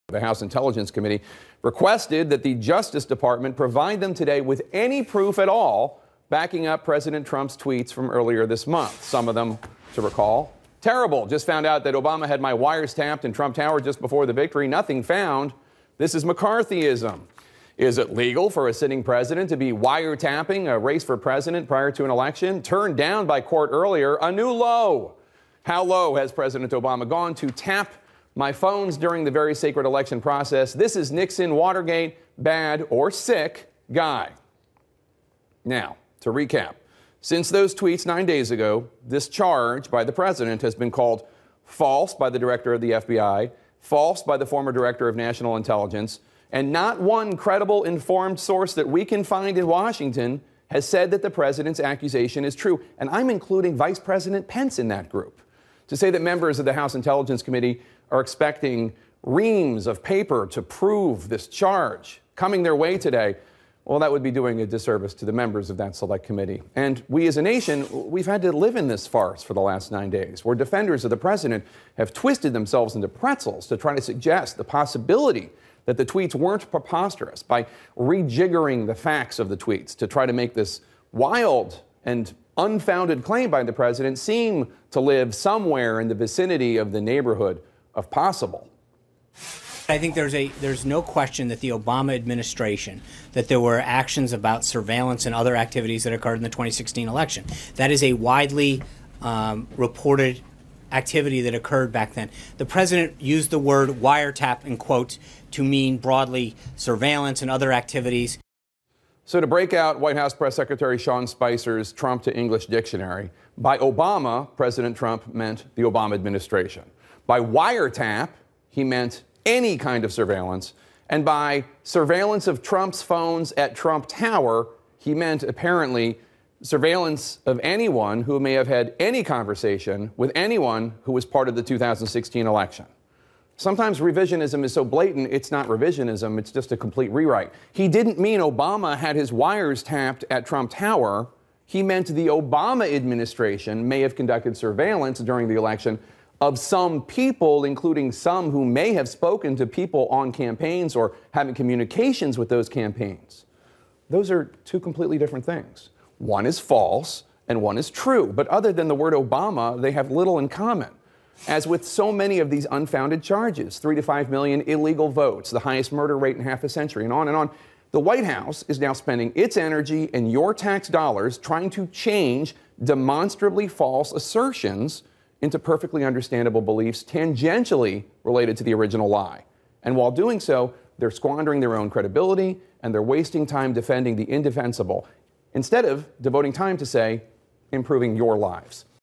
Tapper: We've had to live in this farce
CNN's Jake Tapper said "we as a nation have had to live in this farce," referring to the fallout of President Donald Trump's claims that the Obama administration wiretapped him during the 2016 election.